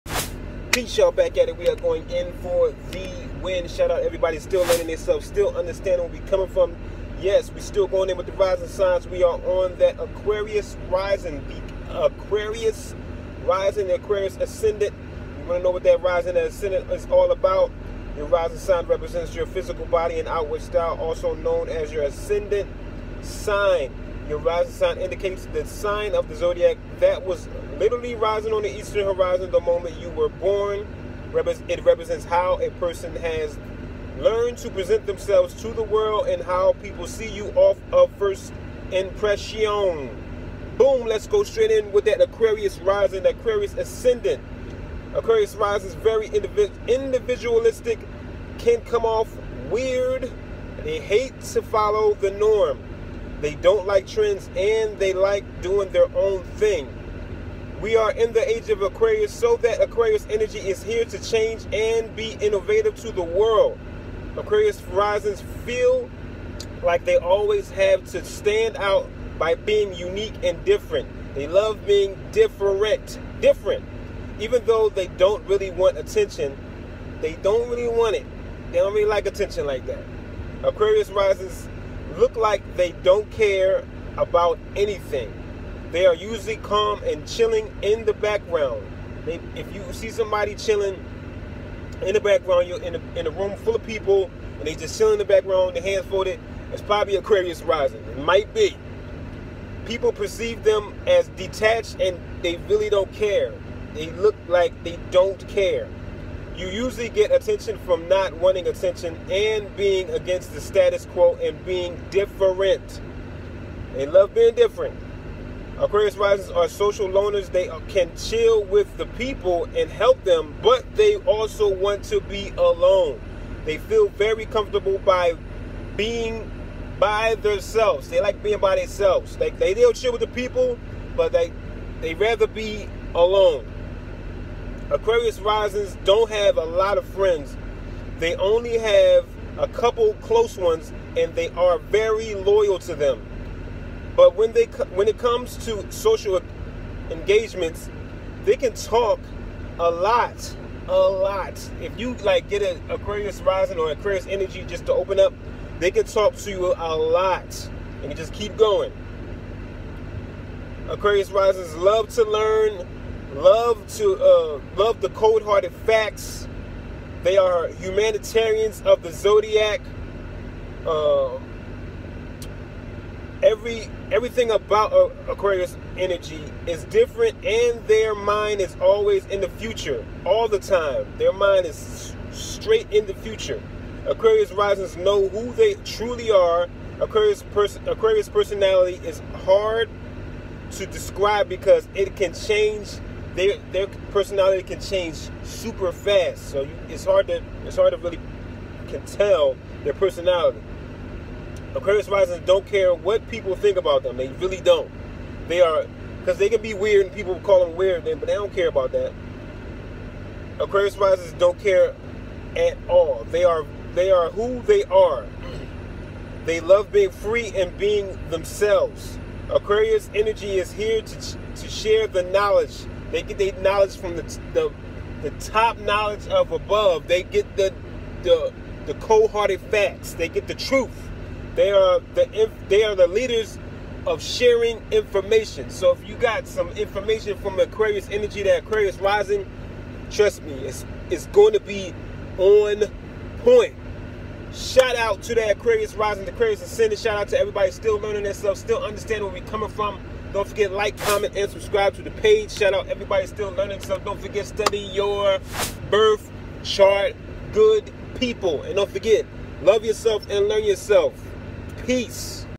Aquarius Rising/Ascendant
The Aquarius rising sign is intelligent, progressive, unique and self-aware. They
are concerned with humanitarian ideals.
These people are often political liberals. They strive to improve society, and fight for justice, economy and humanitarian issues. Aquarius ascendants tend to be awakened and idealistic: their beliefs tend
to be progressive and unconventional. They have a brilliant mind that loves to
learn and discover new things.

Understanding of who we are and where we come from.
We are all made from this “youniverse”
The greatest thing you can learn is you on all levels.